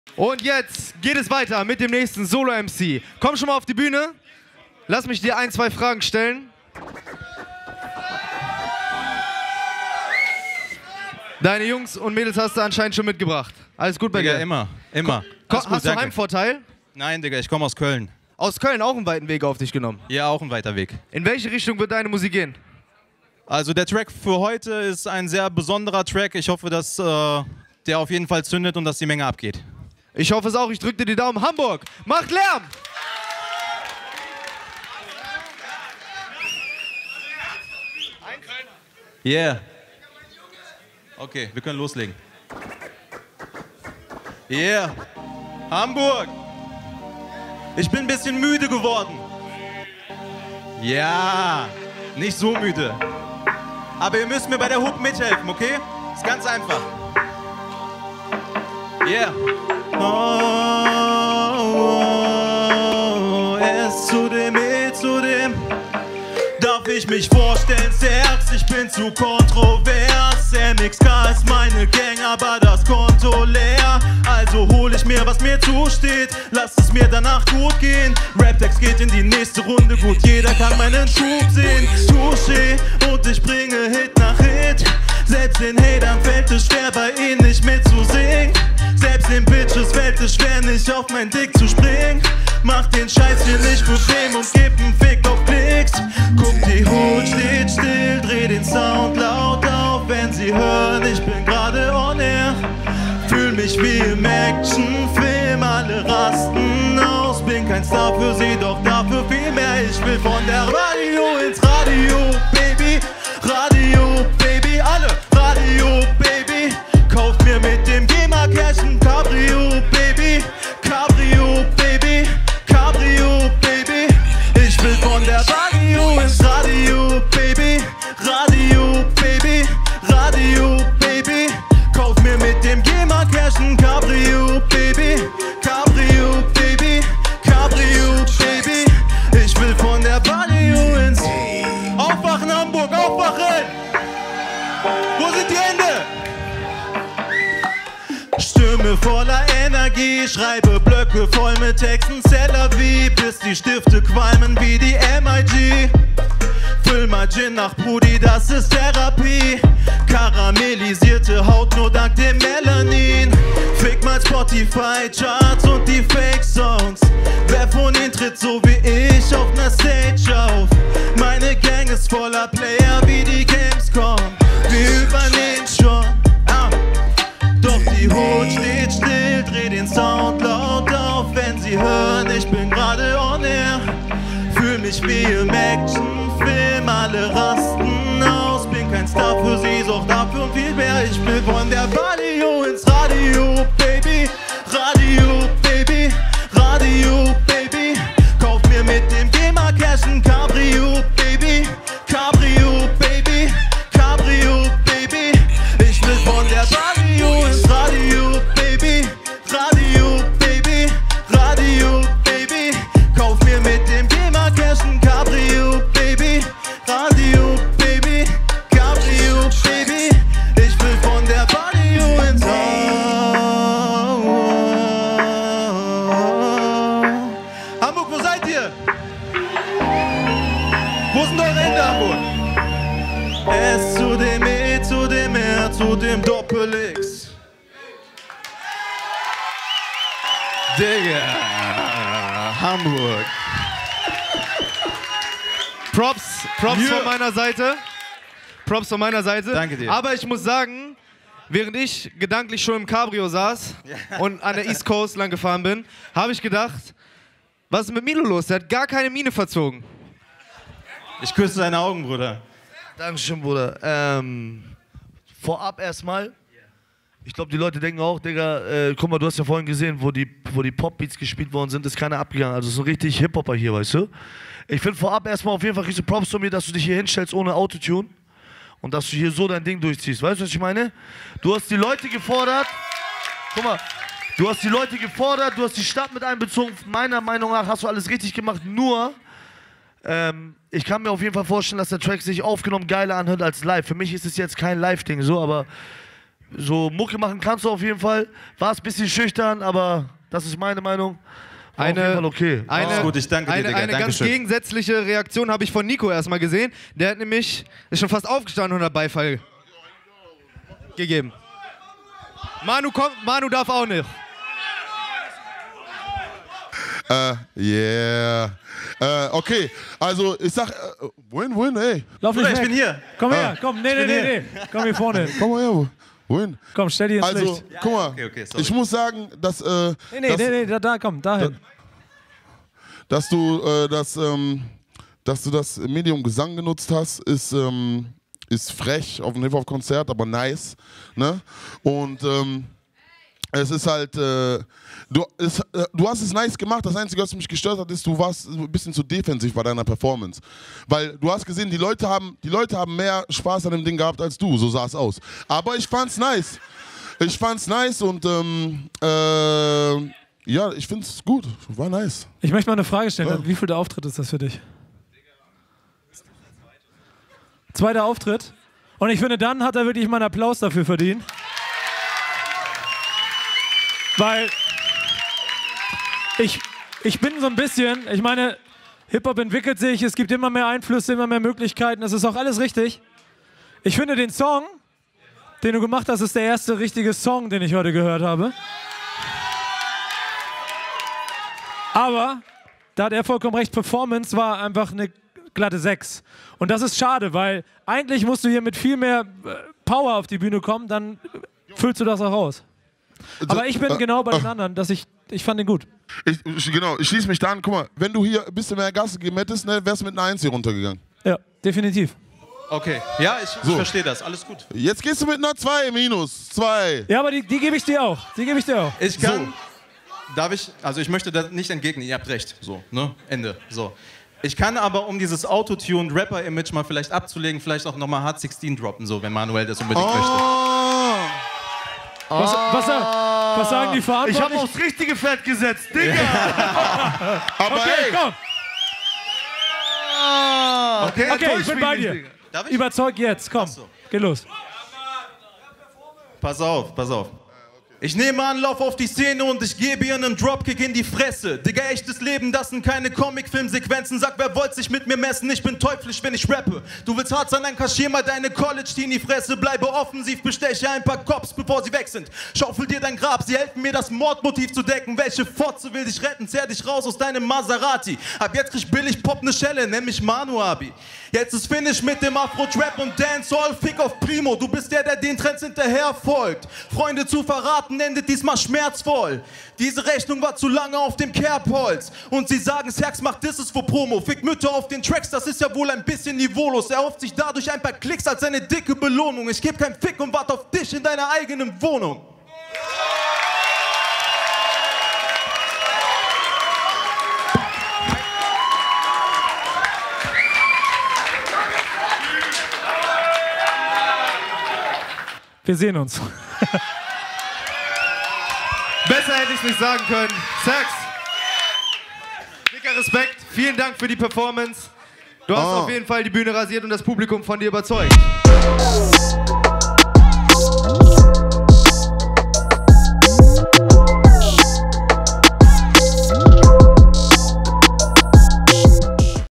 Bühne rasiert mit spontaner Performance: Rapper Serxx bei Raptags2018 in Hamburg
Krasse spontan Performance von Rapper Serxx nach seinem Auftritt!
„Frech aber nice“ urteilt Manuellsen. Unbedingt bis zum Ende anschauen!

Du willst dich bei Raptags bewerben?

Du willst beim Live Finale in Berlin dabei sein?

Video by LBNSWRK